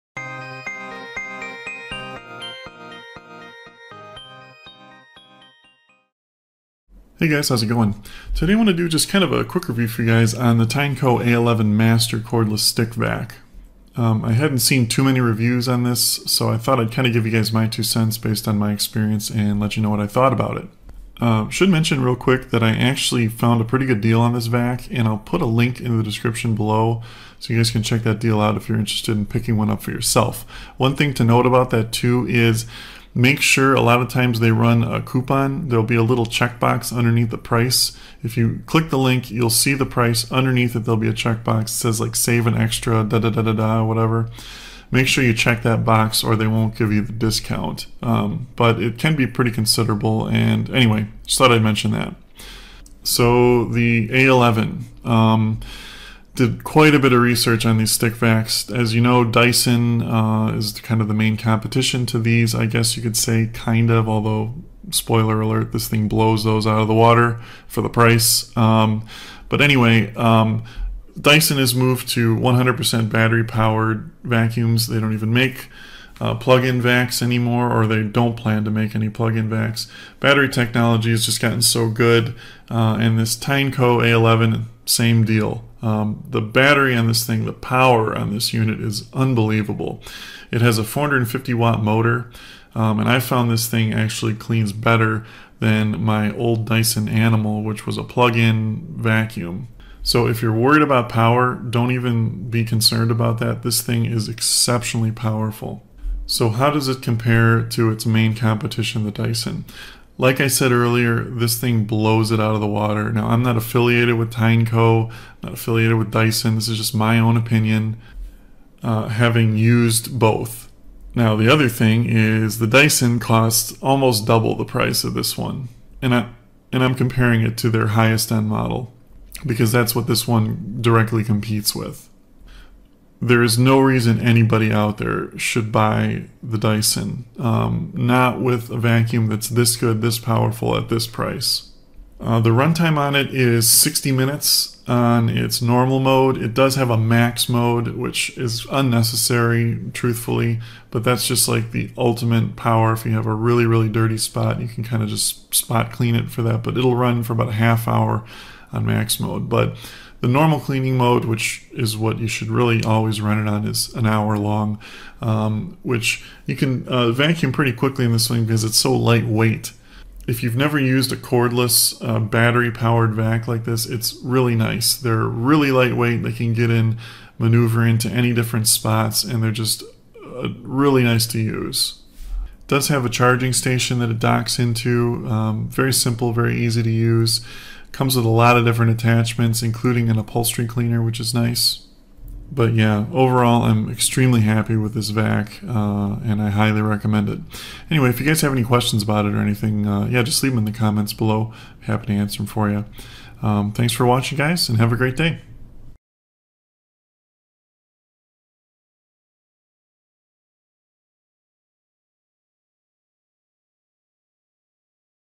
Review Tineco A11 Master Cordless Stick Vacuum 2020
Tineco A11 Master Cordless Stick Vacuum Review

Tineco A11 Master+ Cordless Stick Vacuum Cleaner, Ultra Powerful Suction, Multi-Surface Cleaning, Great for Pet Hair, Emerald Green:

+ High performance cordless vacuum for whole home use featuring a 450W motor and up to 120W of suction power for deep, thorough cleaning on any floor surface.
+ Includes 2 Full Size LED Power Brushes, specifically designed to clean hard floors and carpets. Effectively picks up all types of pet hair.
+ Long-lasting battery delivers up to 50 minutes of uninterrupted runtime. A11 MASTER+ model includes 2 detachable batteries.
+ Easily converts to a handheld vacuum with versatile attachments to reach corners, stairs and crevices. Extra large 0.6L dustbin captures dirt and pet hair.
+ A11 MASTER+ model includes: Full-size LED Multi-tasker Power Brush, Mini Power Brush, (2) Pre-Filters, Pre-Filter Cleaning Tool, 2-in-1 Dusting Brush, Soft Dusting Brush, Crevice Tool, Flexible Long Crevice Tool, Hair Cleaning Tool, Dual Charging Wall-Mounted Dock --- PLUS Full-size LED Soft Roller Power Brush.